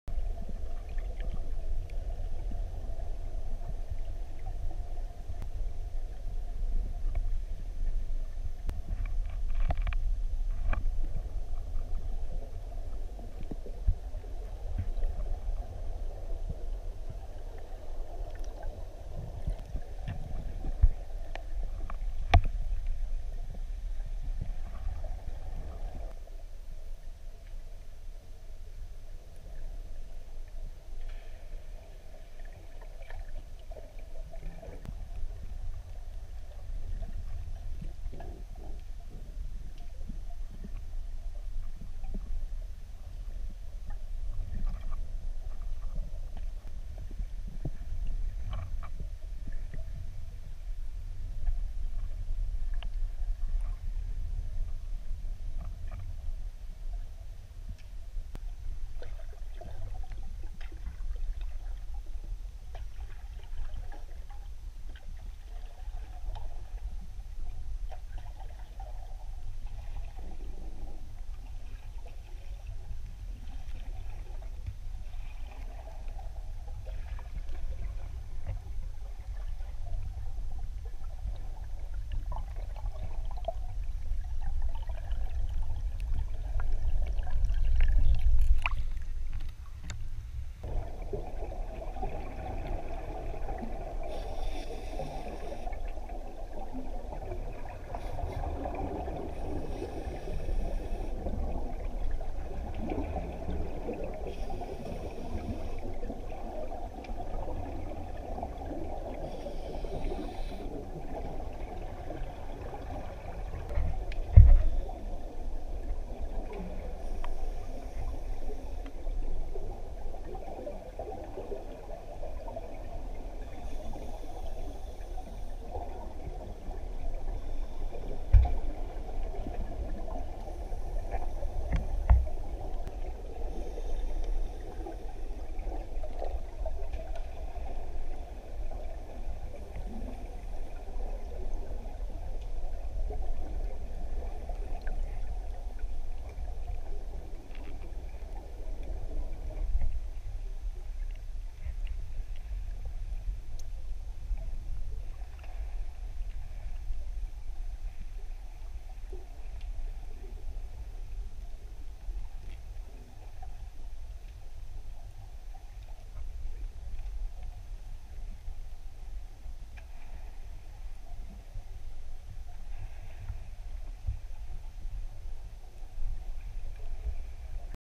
Freediving at Innerkip Quarry.
I spent couple hours discovering cars, plane and bus at Trout Lakes in Innerkip on June 19, 2016